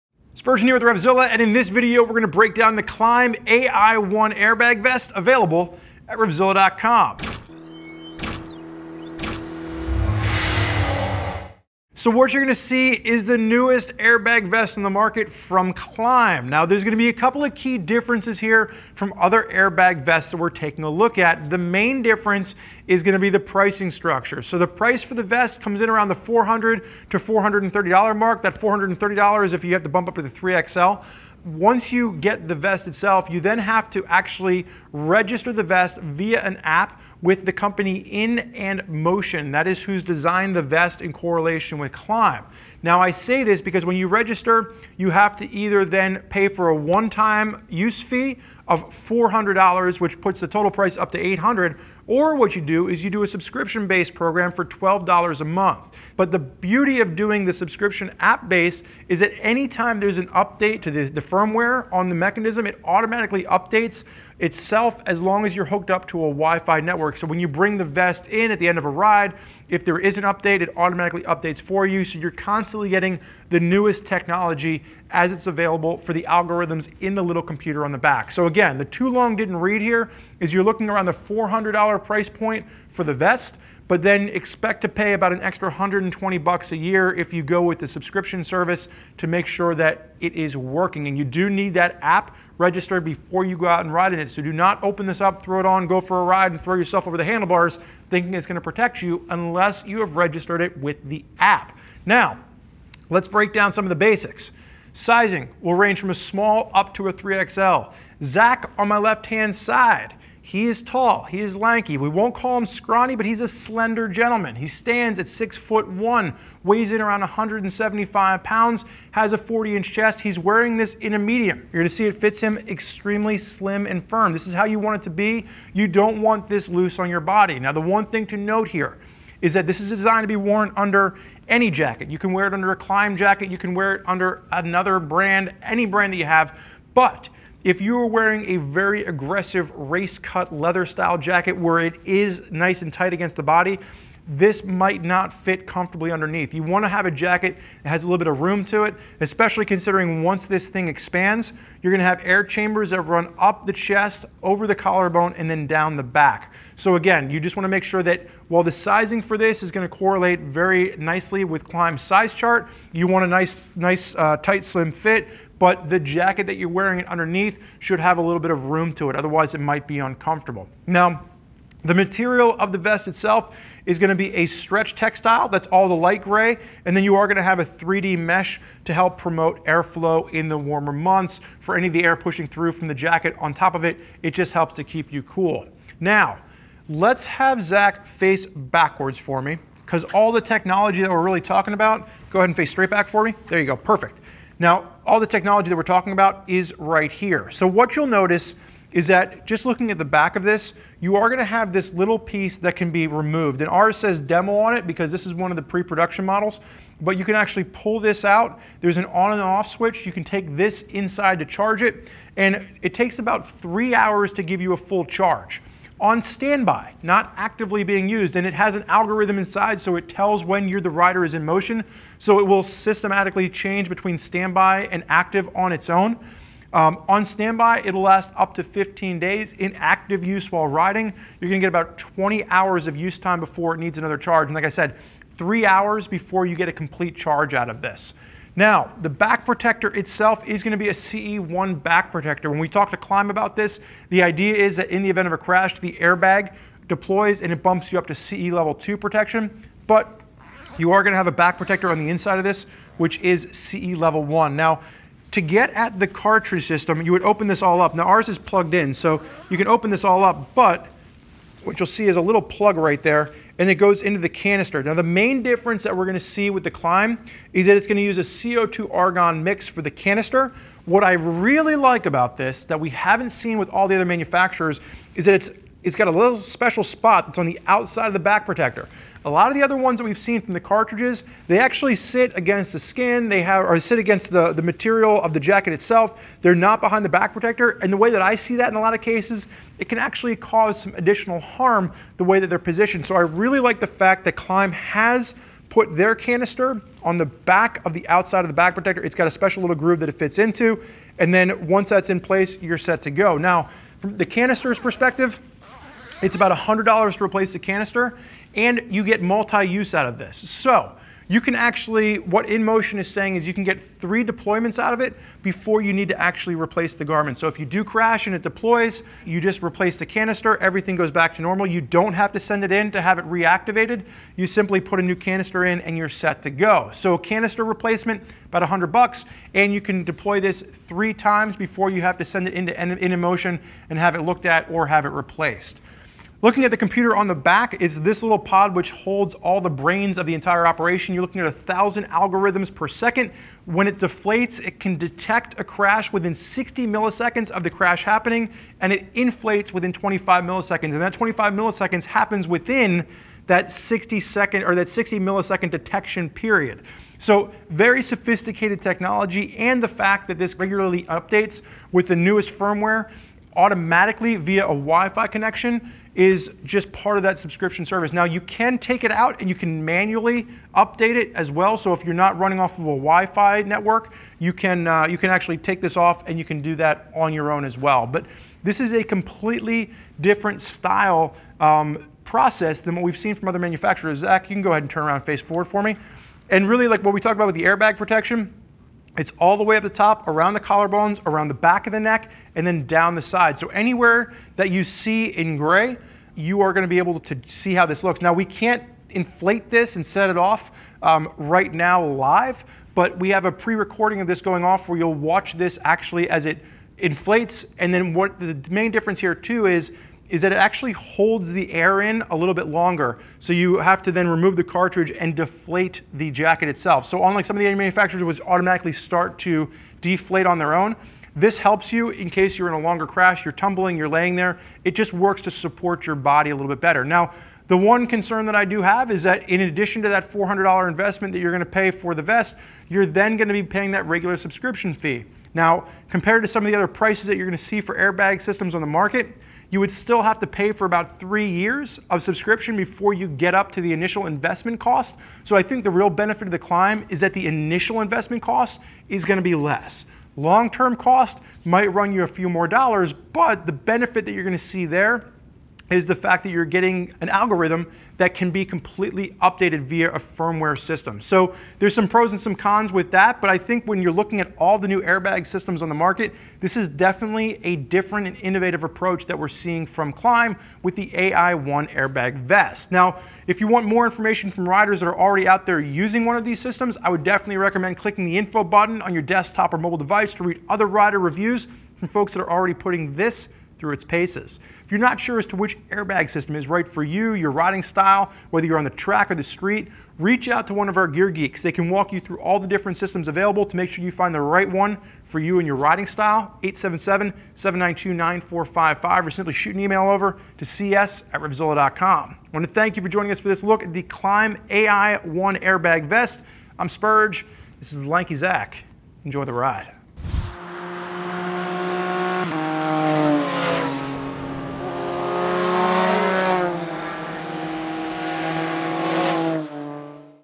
Klim Ai-1 Airbag Vest Review
Get ready for action with the Klim Ai-1 airbag vest. Klim has constructed the Ai-1 with the same careful attention to detail that they bring to their other products, resulting in a technologically-advanced package. Fully wireless and tether-free, the Ai-1 can detect and inflate in under 60 milliseconds. Operating on subscription-based In&Motion, the Ai-1 connects with your smartphone. The Ai-1 protects the rider’s thorax, abdomen, spine and neck. Additional features include user-replaceable cartridges (3 deployments possible before the airbag needs a technical evaluation), 20+ hours of continuous battery life, 3 hour charge time, and an Integrated CE 2 certified static back protector. The Klim Ai-1 has a 5-star SRA rating and certified to CE prEN 17092-6. The Ai-1 is not intended for use off-road.